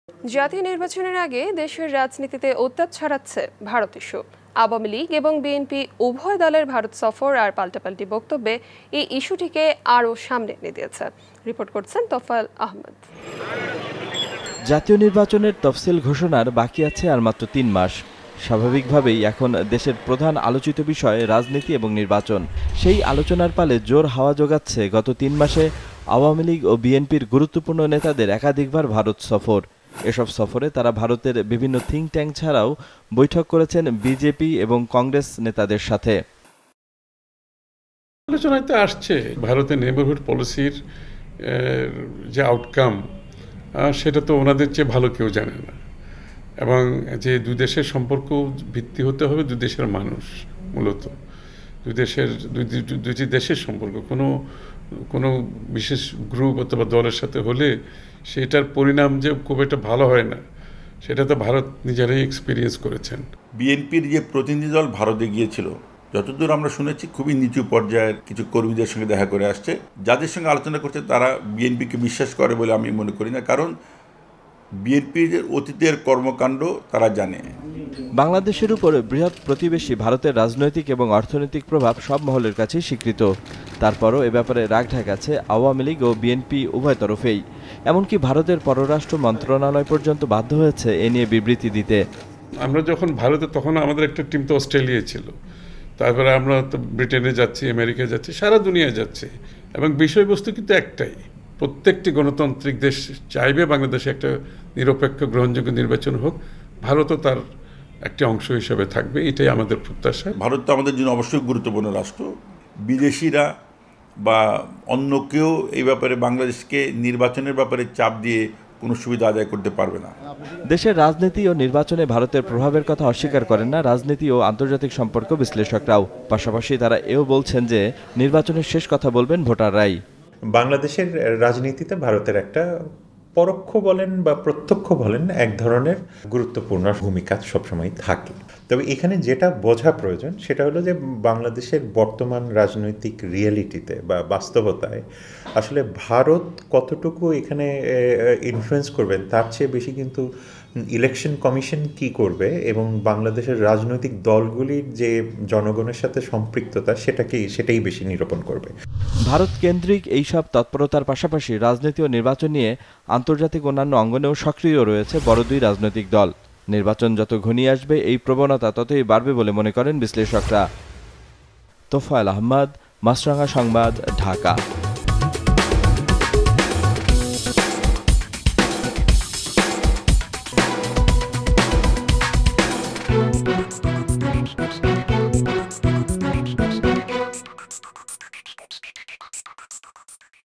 নির্বাচনের আগে উত্তাপ ছড়াচ্ছে ভারত ইস্যূ
জাতীয় নির্বাচনের আগে দেশের রাজনীতিতে উত্তাপ ছাড়াচ্ছে ভারত ইস্যূ। আওয়ামী লীগ এবং বিএনপি উভয় দলের ভারত সফর  আর পাল্টাপাল্টি বক্তব্যে এ ইস্যুটিকে আরও সামনে এনে দিয়েছে।